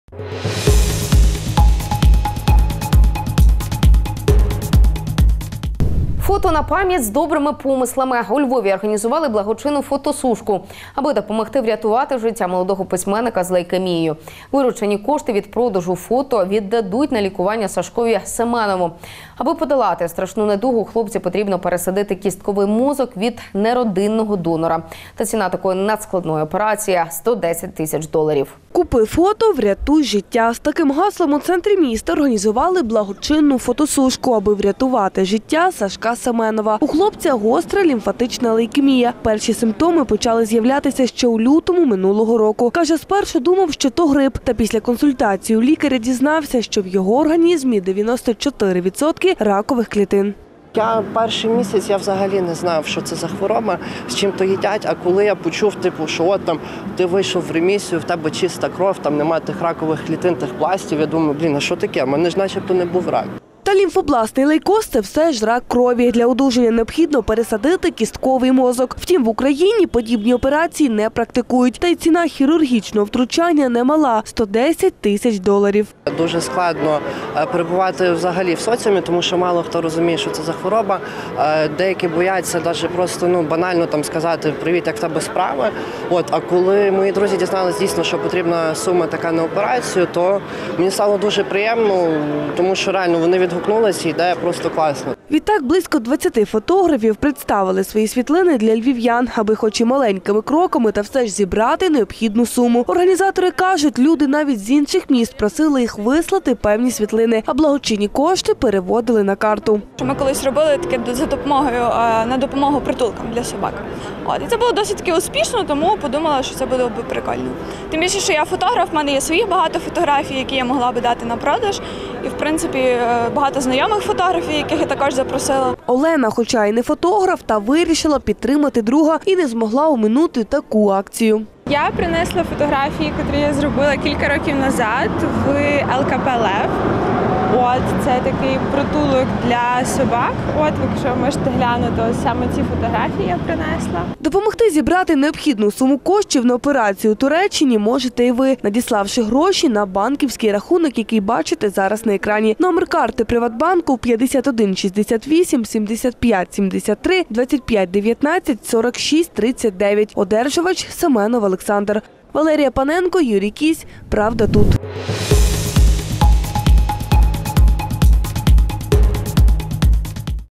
Фото на пам’ять з добрими помислами. ПравдаТУТ Львів
Фото на пам'ять з добрими помислами. У Львові організували благочинну фотосушку, аби допомогти врятувати життя молодого письменнника з лейкемією. Виручені кошти від продажу фото віддадуть на лікування Сашкові Семенову. Аби подолати страшну недугу, хлопцю потрібно пересадити кістковий мозок від неродинного донора. Та ціна такої надскладної операції - 110 тисяч доларів.